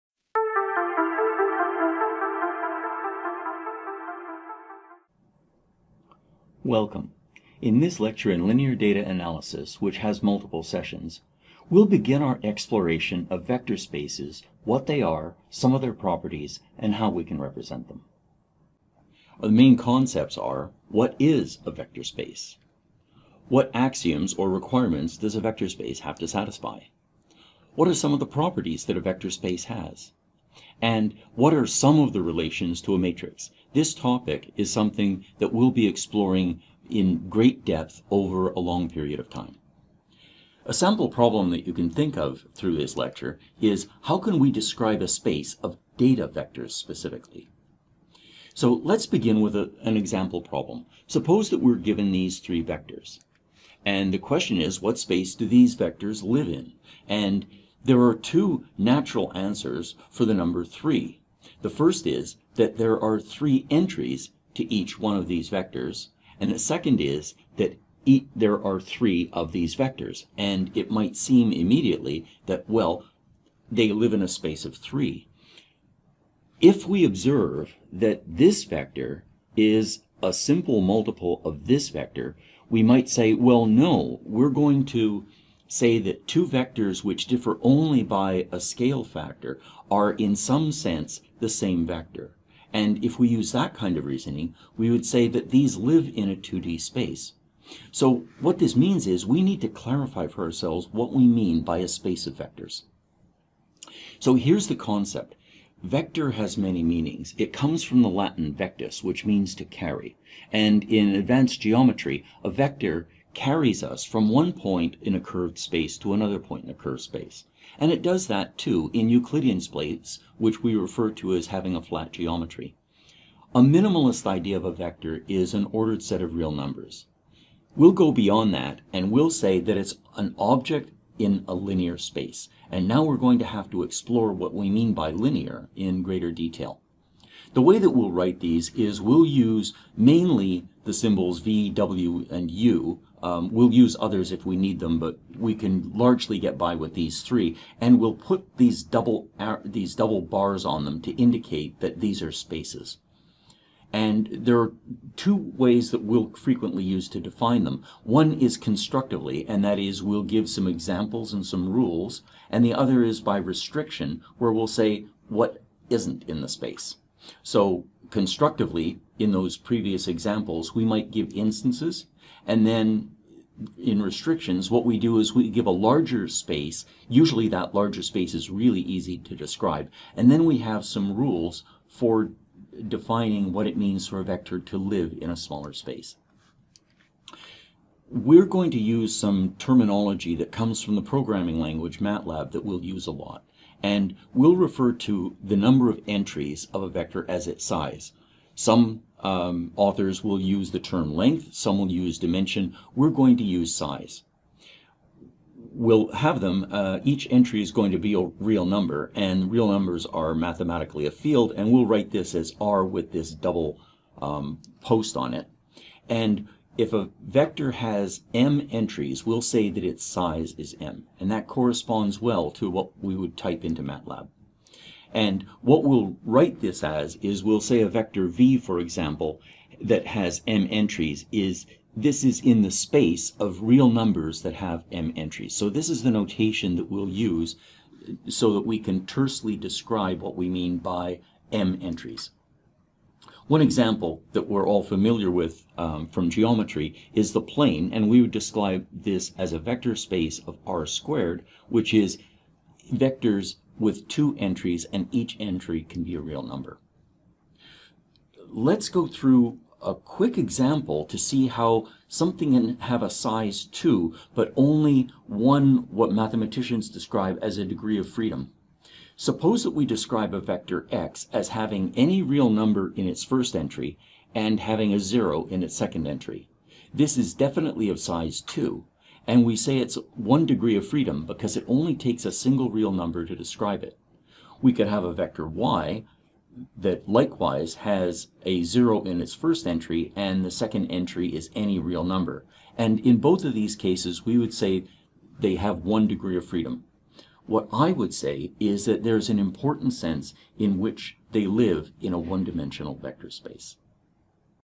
LinearData04aVSpaceIntro
Introduction to vectors and vector spaces; where vectors "live in". Part of a series of lectures: